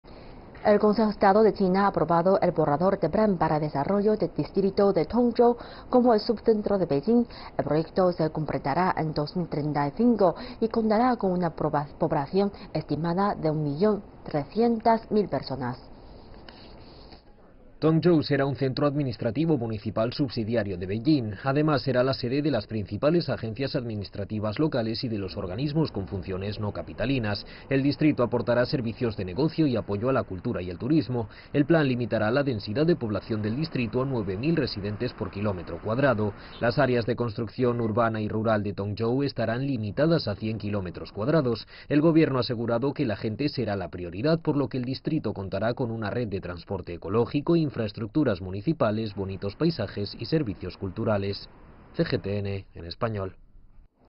Consejo de Estado aprueba el plan para el subcentro de Beijing
El Consejo de Estado de China ha aprobado el borrador del plan para el desarrollo del distrito de Tongzhou, como el subcentro de Beijing. El proyecto se completará en 2035 y contará con una población estimada de un millón 300 mil personas.